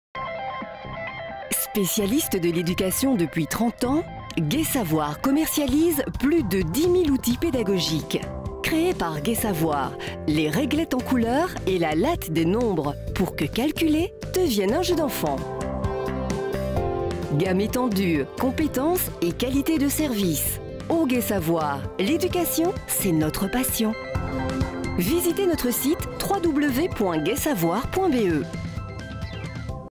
Gai Savoir - Réglettes des nombres
Les nombres en couleurs
Pour que calculer devienne un jeu d'enfant !
Ce matériel, mis au point par Gai Savoir, est l'aboutissement de 60 ans de pratique dans les écoles, de la méthode de Georges Cuisenaire.